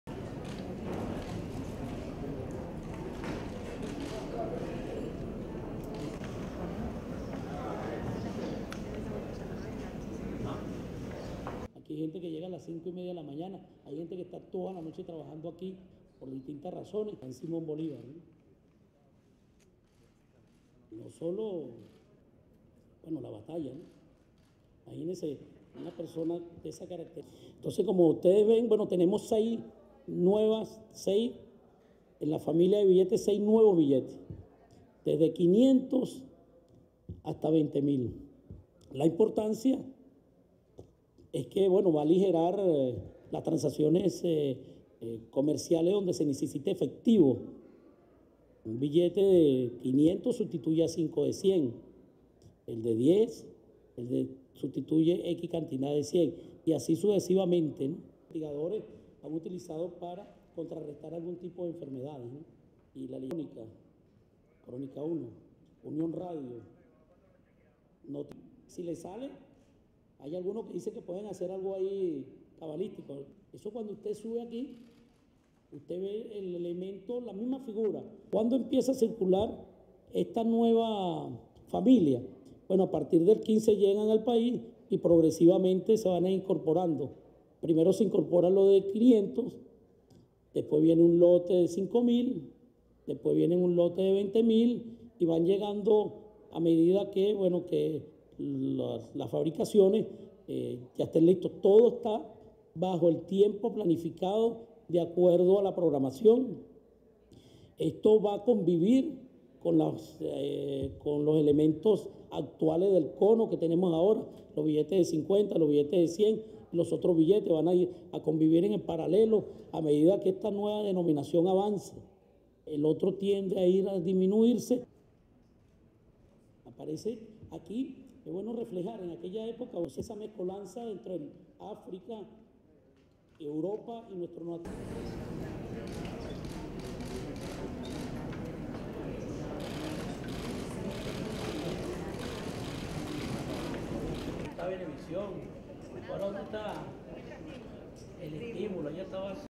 Venezuela: Six new bills to go into circulation
(7 Dec 2016) Venezuela has issued six new bills set to go into circulation next week.
Central Bank President Nelson Merentes said Tuesday the new higher-denomination bills would make commercial transactions easier in the country with the world's highest inflation.
The six new bills ranging from 500 to 20,000 Bolivars will begin circulating on Dec. 15.
Triple-digit inflation and a currency meltdown have left the country's largest note worth just around 2 US cents on the black market. Currently the largest-denominated bill is 100 bolivars, not even enough to buy a hard candy at a street kiosk.
Some had speculated that the new bills might feature late socialist President Hugo Chavez, or other nods to left-wing causes.
But they instead portray the same historical figures and national heritage as existing bills.